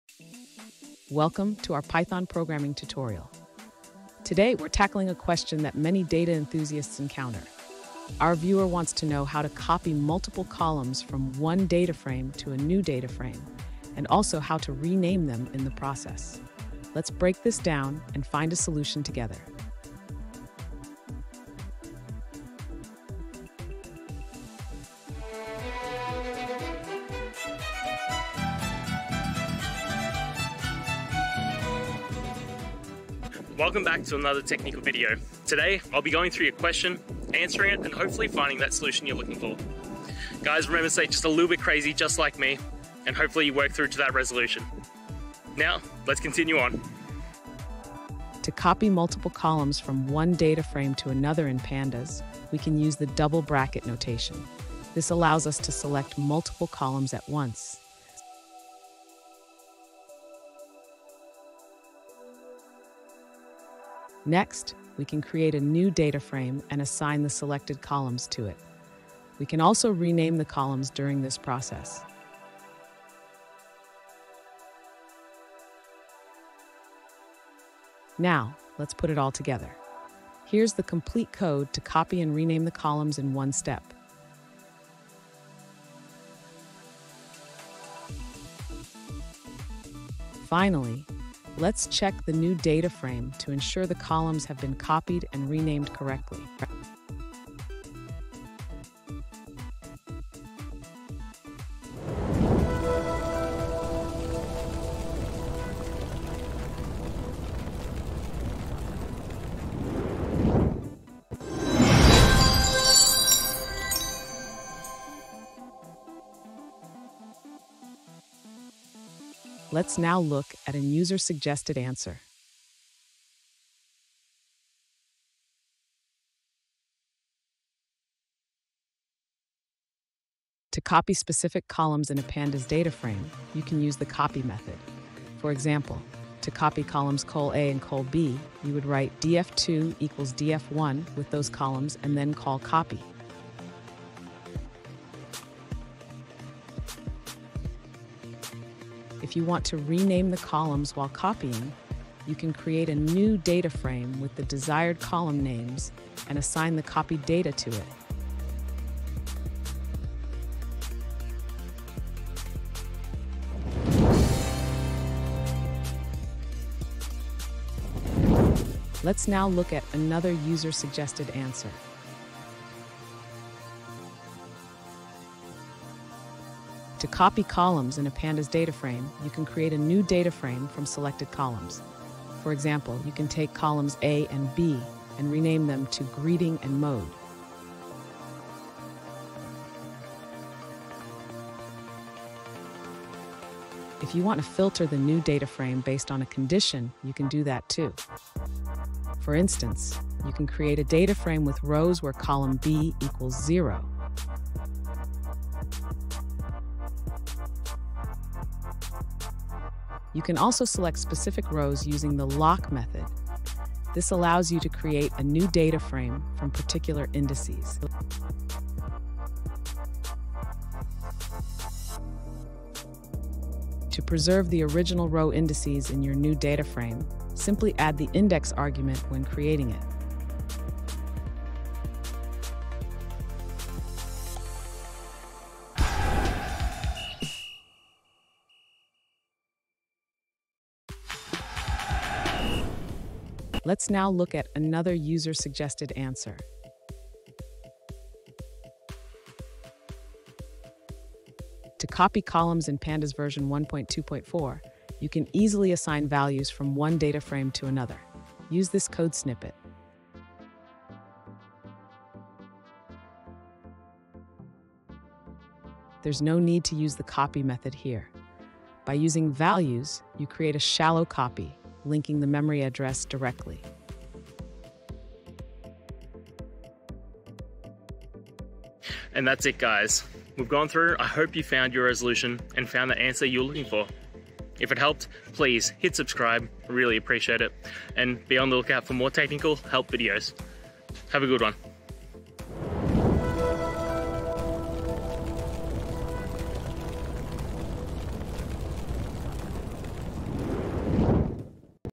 How to Copy Columns in Python Pandas: A Step-by-Step Guide
In this video, we’ll explore the powerful capabilities of Python's Pandas library, specifically focusing on how to efficiently copy columns within a DataFrame. Whether you're a beginner looking to understand the basics or an experienced user seeking to refine your skills, this step-by-step guide will walk you through the process, providing clear examples and practical tips. Join us as we unlock the potential of data manipulation with Pandas!

Today's Topic: How to Copy Columns in Python Pandas: A Step-by-Step Guide

Related to: python, pandas, copycolumns, datamanipulation, step-by-stepguide,